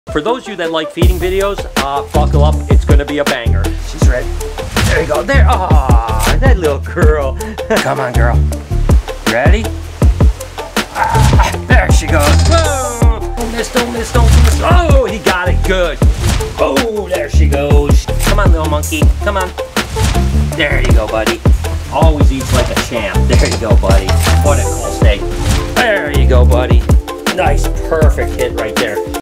SNAKE FEEDING FRENZY!!! BRIAN BARCZYK
It's a food frenzy! Time to eat! Feeding a bunch of my snakes at The Reptarium!

↑↑↑↑↑↑FOLLOW NOAH↑↑↑↑↑↑